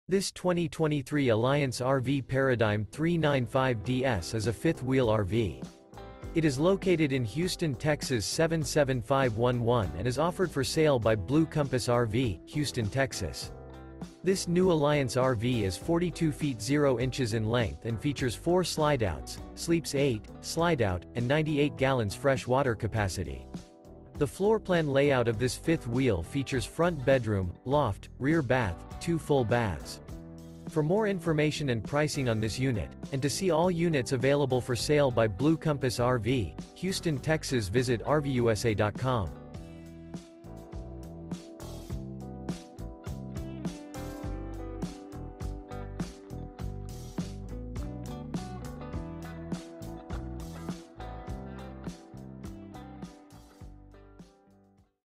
Phenomenal 2023 Alliance RV Paradigm Fifth Wheel RV For Sale in Houston, TX | RVUSA.com
This 2023 Alliance RV Paradigm 395DS is a Fifth Wheel RV. It is located in Houston, TX 77511 and is offered for sale by Blue Compass RV - Houston, TX. This New Alliance RV Is 42' 0" in length and features 4 slideouts, sleeps 8, Slideout, and 98 gal fresh water capacity. The floorplan layout of this Fifth Wheel features Front Bedroom, Loft, Rear Bath, Two Full Baths.